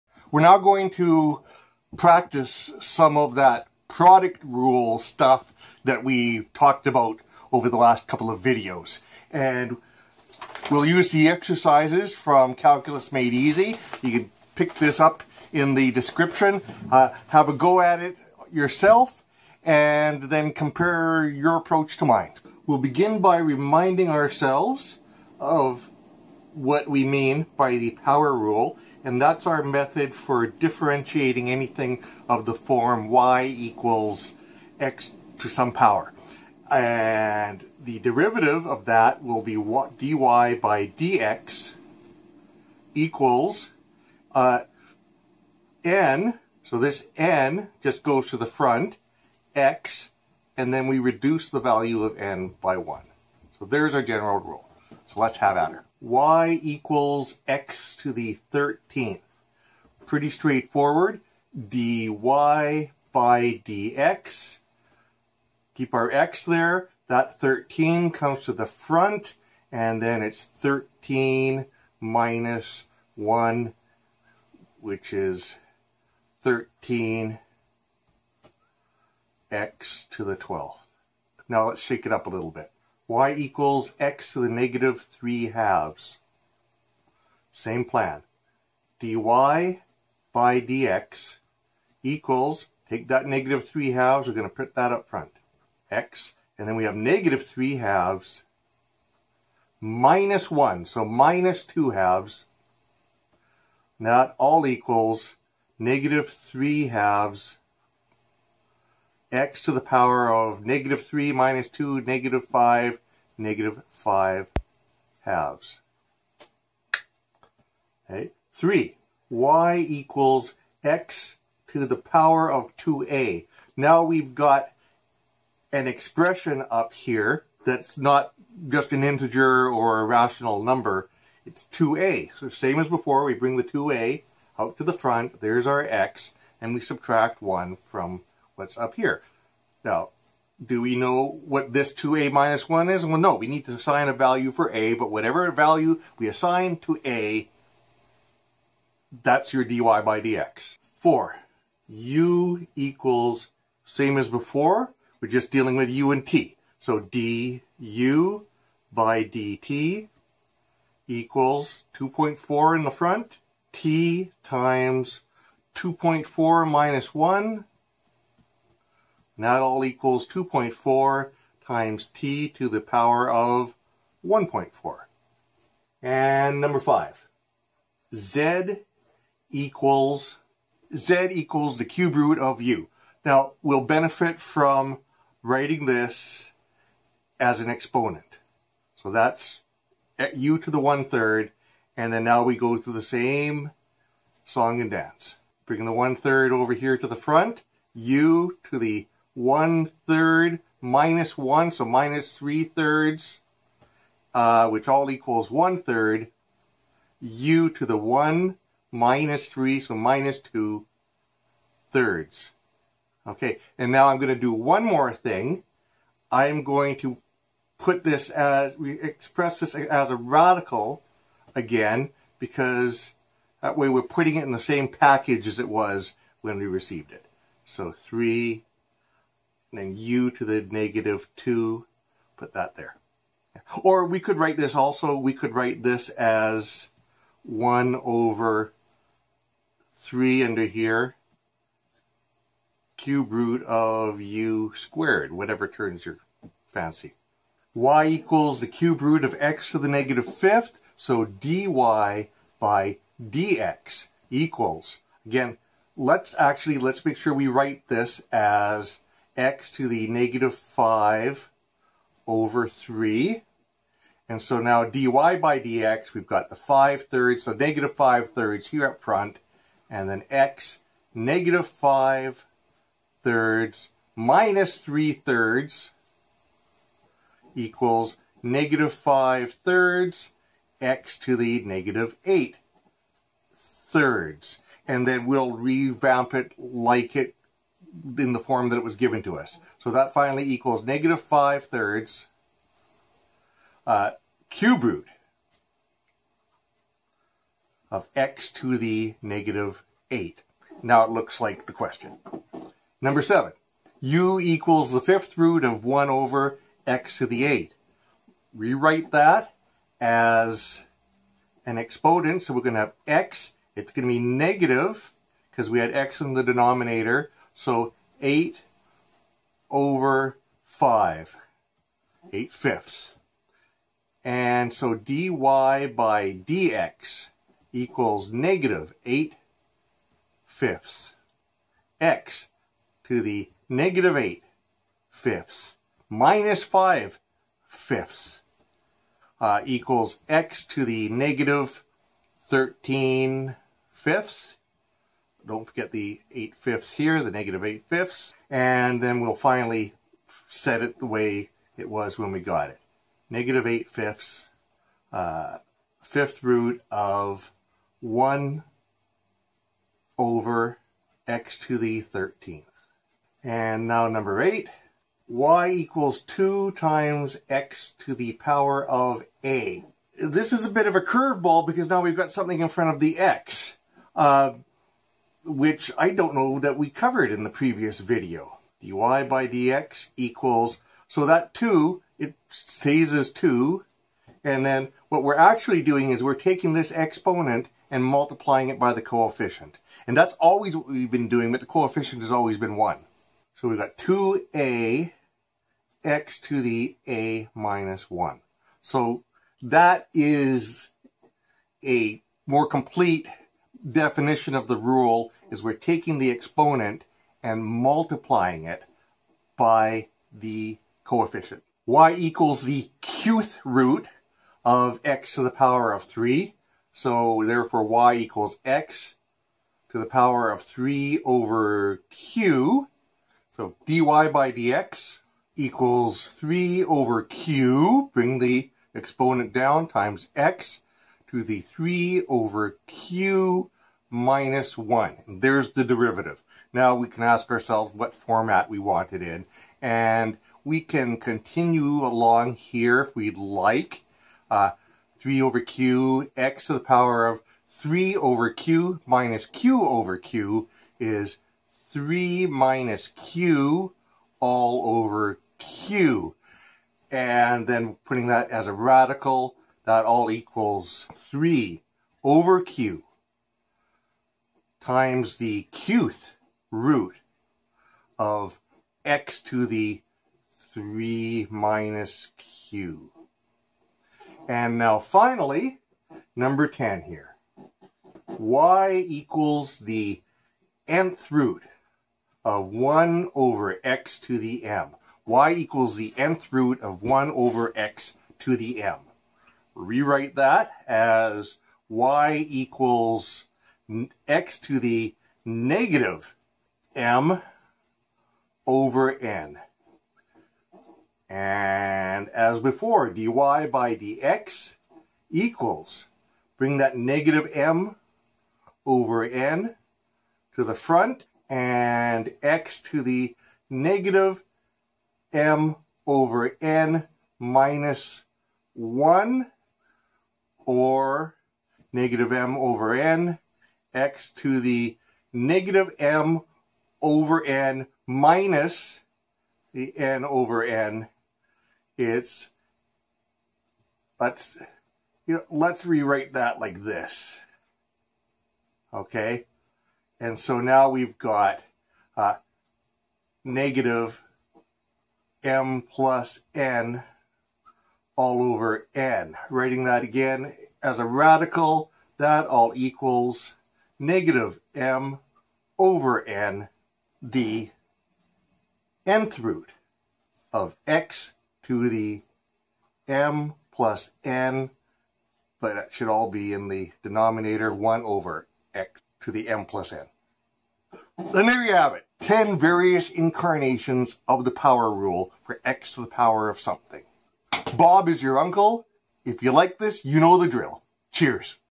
10 Derivatives in 10 minutes (or so)--Power Rule Practice
Calculus Made Easy, Chapter 4. Have a crack at the 10 Power Rule practice exercises
Compare your results to the ones in the video. How did you do?
0:23 Power Rule Recap
0:15 y=x^13
0:23 y=x^(-3/2)
1:58 y=x^(2a)
2:30 u=t^(2.4)
2:57 z=cuberoot(u)
4:07 y=cuberoot(x^(-6))
5:02 u=5thRoot(1/(x^8))
6:03 y=2x^a
6:53 y=q'thRoot(x^3)
7:57 y=n'thRoot(1/x^m)